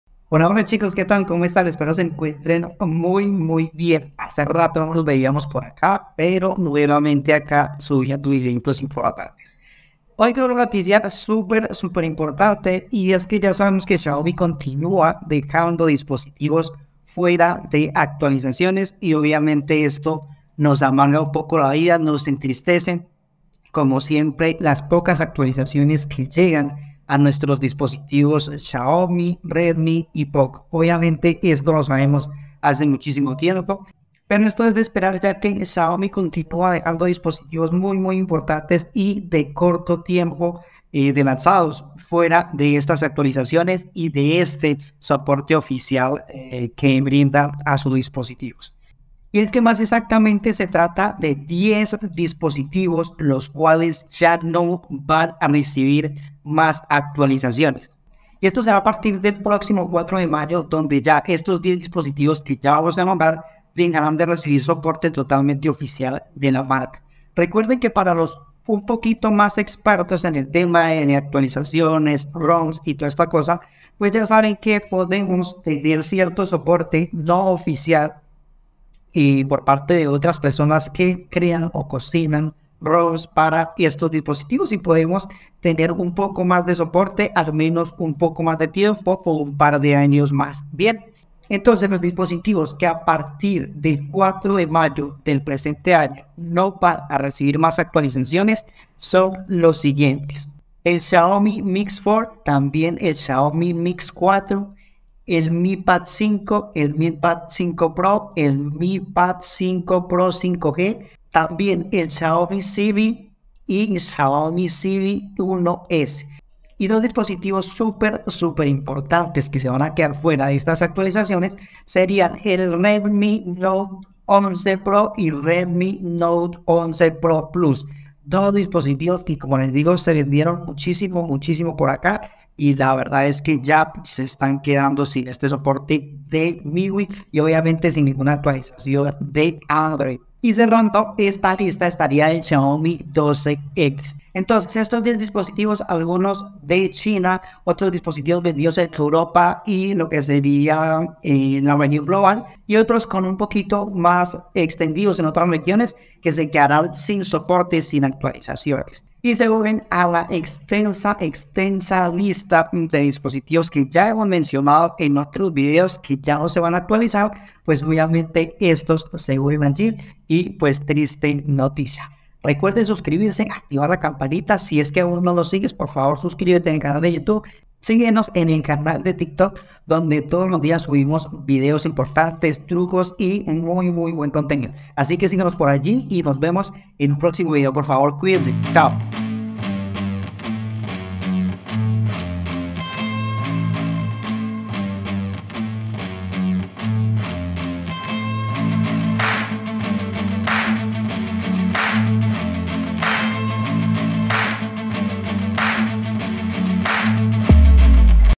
😡 SIN ACTUALIZACIONES 10 DISPOSITIVOS MAS. 🤯
SON EN TOTAL 10 LOS DISPOSITIVOS QUE XIAOMI DEJA POR FUERA DE PROXIMAS ACTUALIZACIONES, LOS CUALES SE UNEN A LA EXTENSA LISTA DE DIAPOSITIVOS SIN SOPORTE.

acá te tengo la lista

NUESTRO FACEBOOK.

RECUERDA QUE ESTAMOS HABLANDO DEL REDMI NOTE 8 2021
Y ESTA ACTUALIZACION ES LA EUROPEA.

CANALES DE TELEGRAM RECOMENDADOS:

CUALQUIER XIAOMI.

REDMI NOTE 8- 8 T.
redminote8globalTecno

POCO M3.